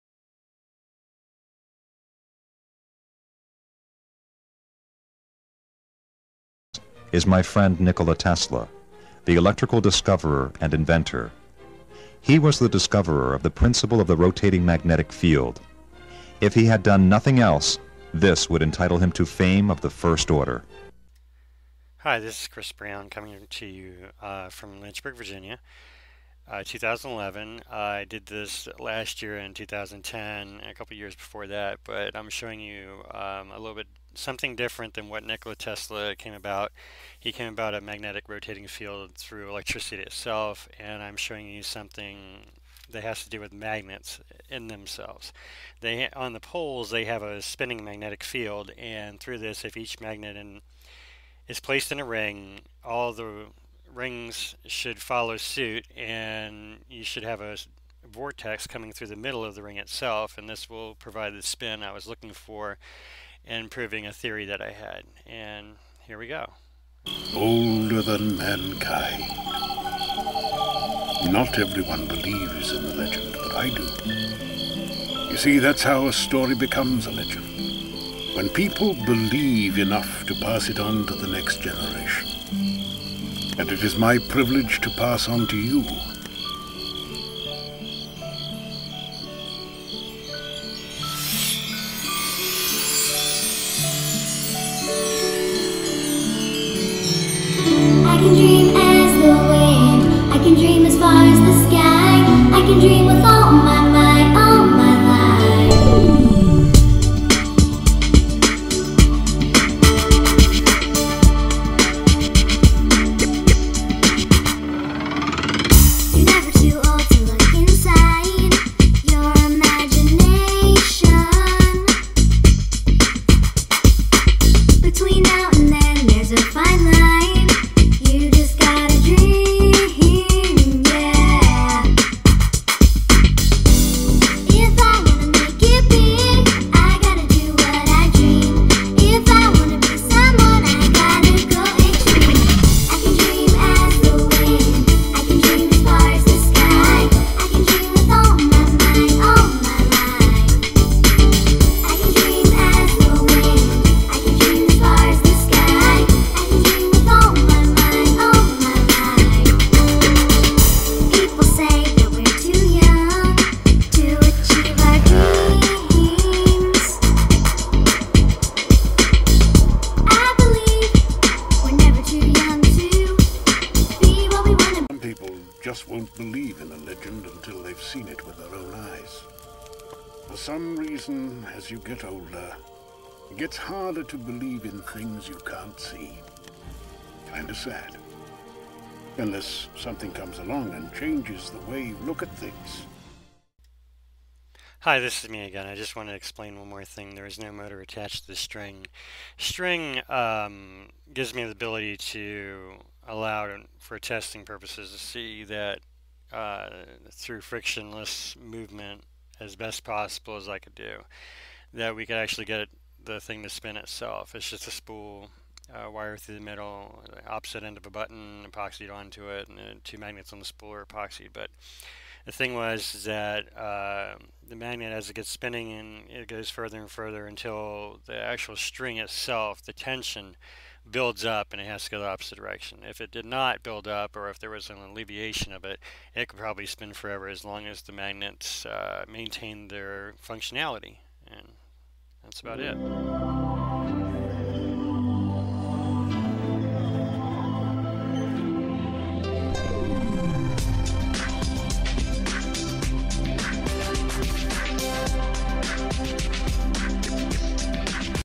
Magnet Clock Commentary Included
A small test of Magnetic Energy. Yes it's real and does work. Comentary with this video.